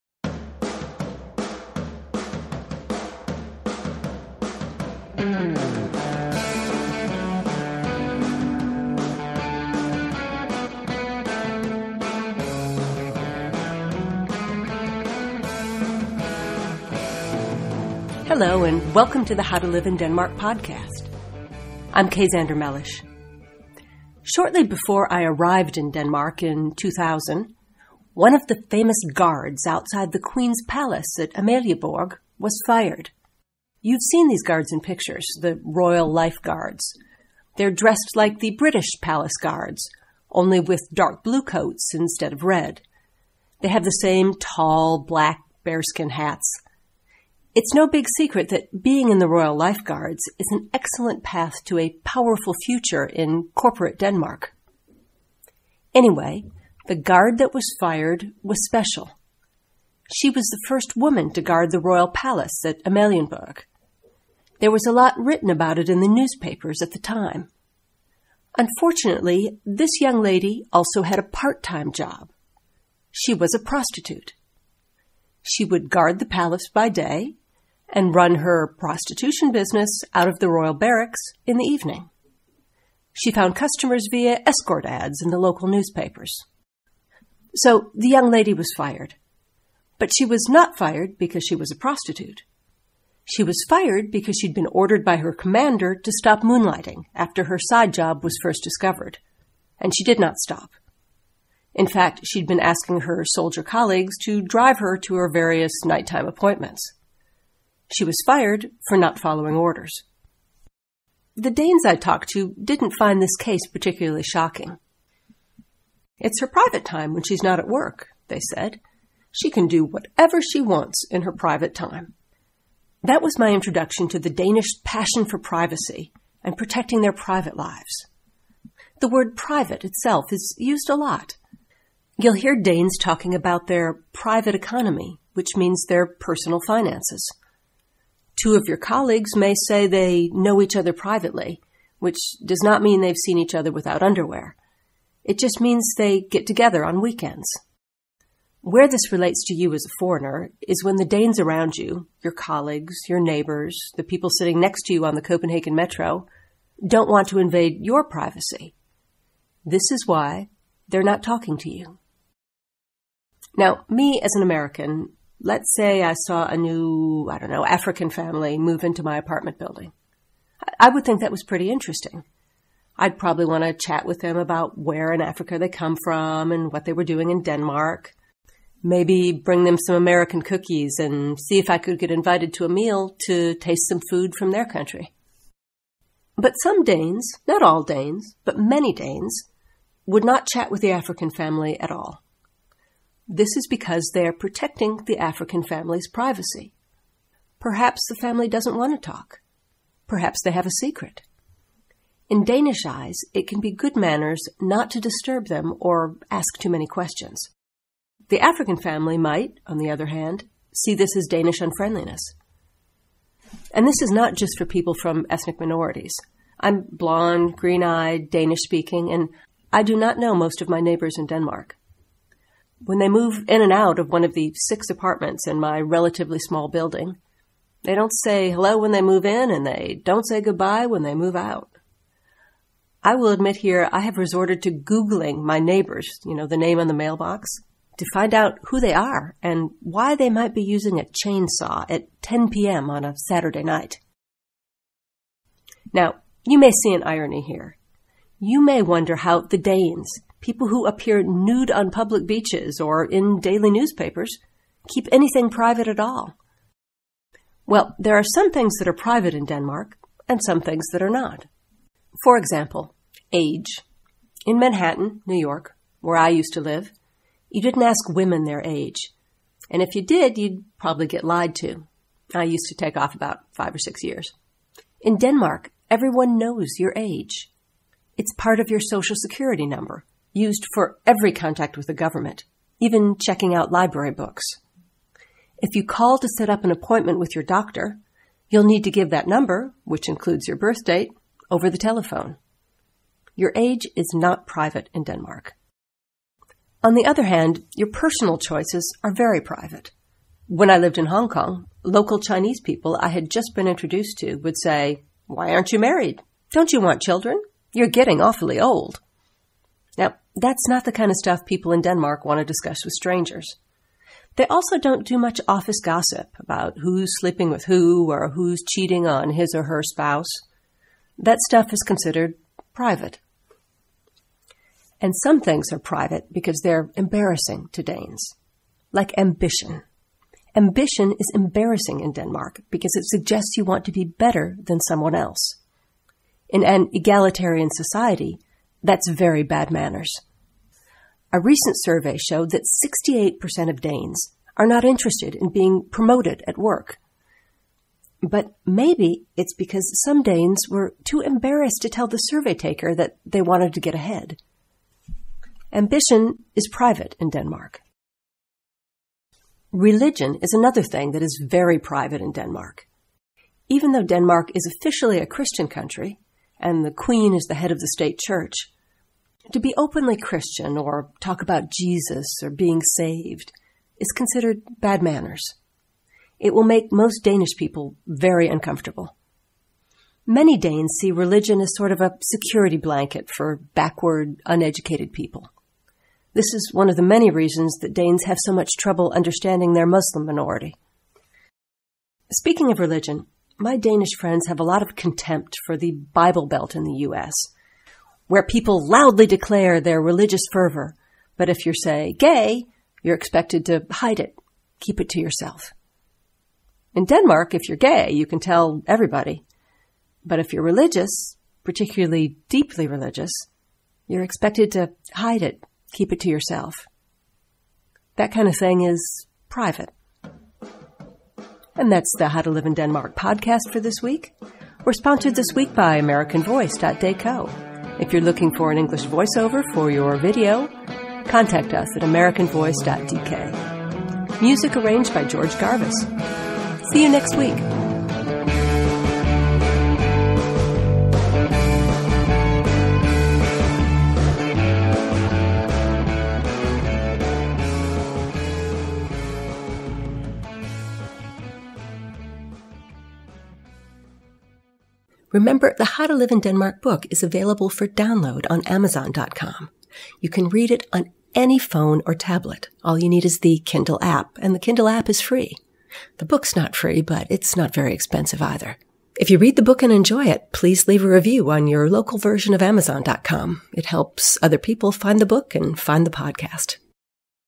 Public nudity and the passion for privacy: Why I Google my Danish neighbors to find out who they...
They may appear nude on public beaches and in daily newspapers, but the Danes have a passion for privacy. And they may be respecting YOUR privacy by not talking to you.

This is the seventh episode of the "How to Live in Denmark podcast", and originally ran in fall 2013.

Get all of Kay Xander Mellish's books on Denmark Book Kay for a talk to your group or organization